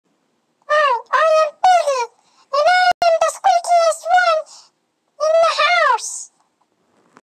My talking Piggy the guinea pig(READ DESCRIPTION)
She said," Hi I'm piggy and I'm the squeakiest one in the house!"